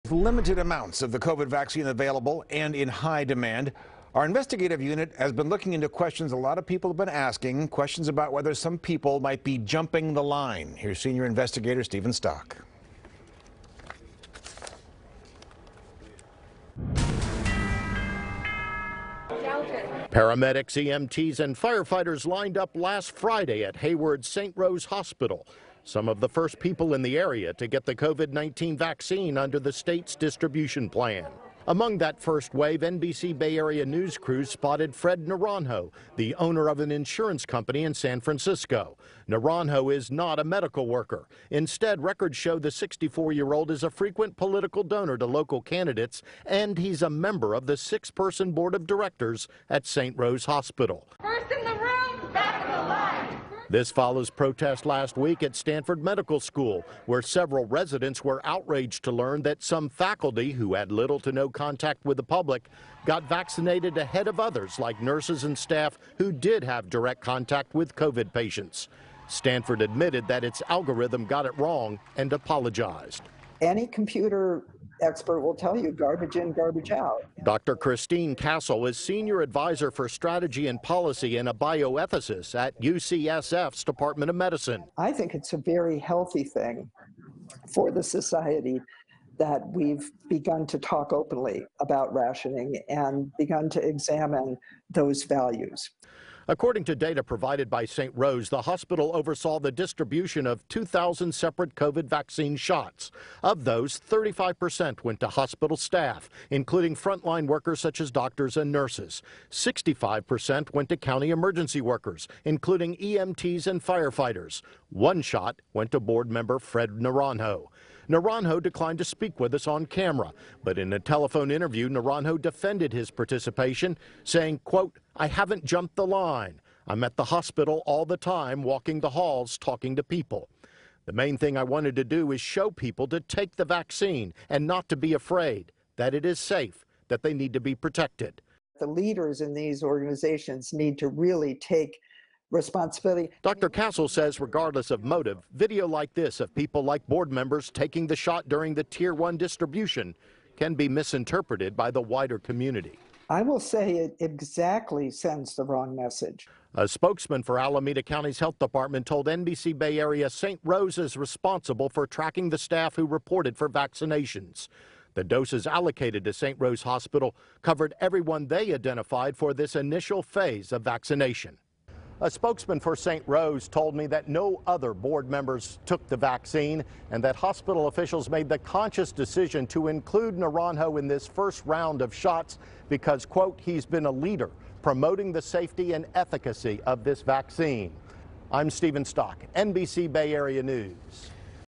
Business Owner on St. Rose Hospital Board Among the First to Get COVID-19 Vaccine
Among the paramedics, firefighters and other first responders lined up to receive the COVID-19 vaccine last Friday at Hayward’s St. Rose Hospital, was one person whom you likely wouldn’t expect to see, according to state guidelines governing vaccine distribution. NBC Bay Area Investigative Reporter Stephen Stock has the story.